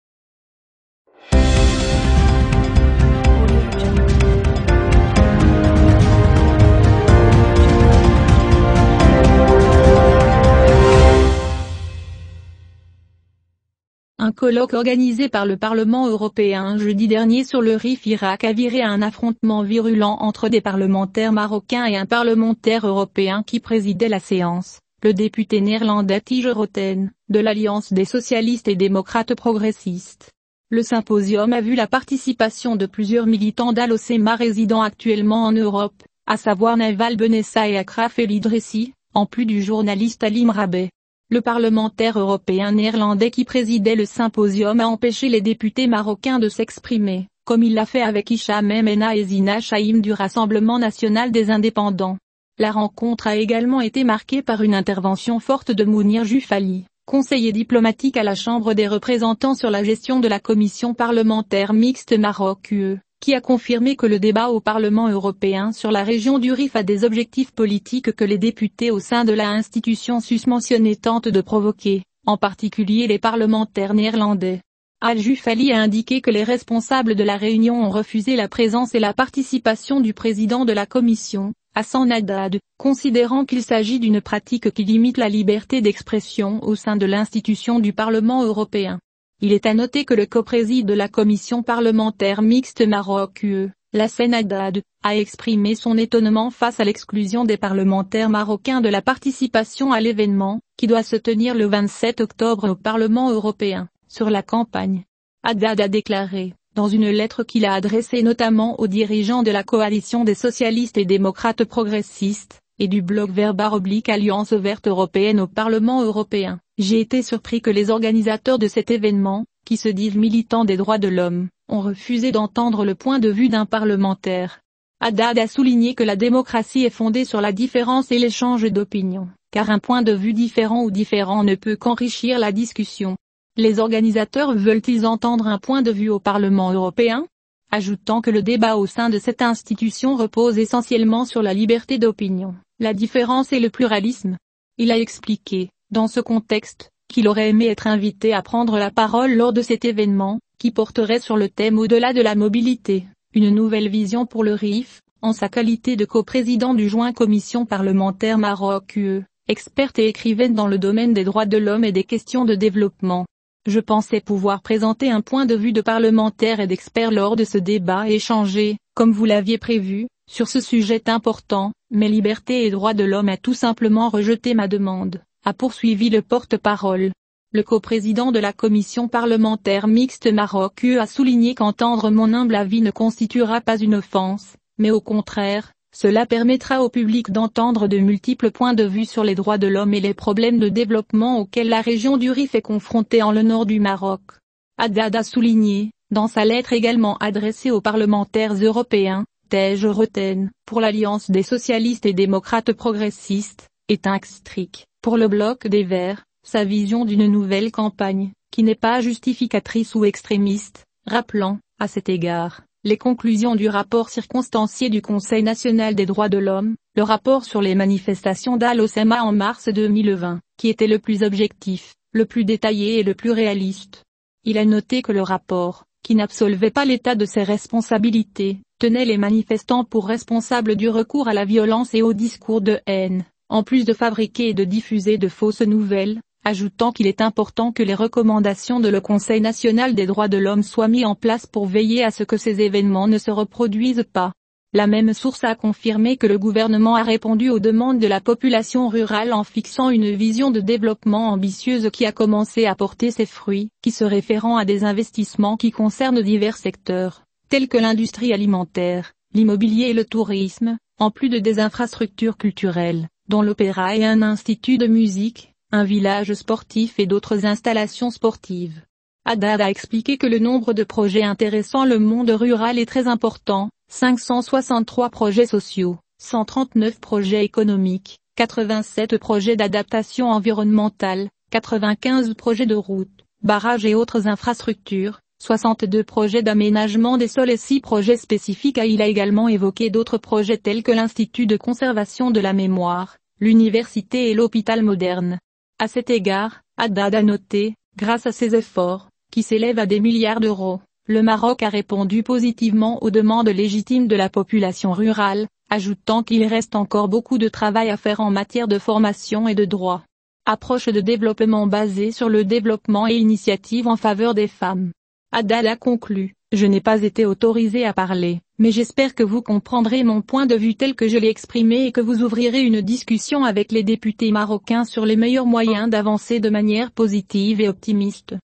Vives confrontations entre députés marocains et européens au Parlement européen à Bruxelles
Vives confrontations entre députés marocains et européens au Parlement européen à Bruxelles en raison du mouvement du Rif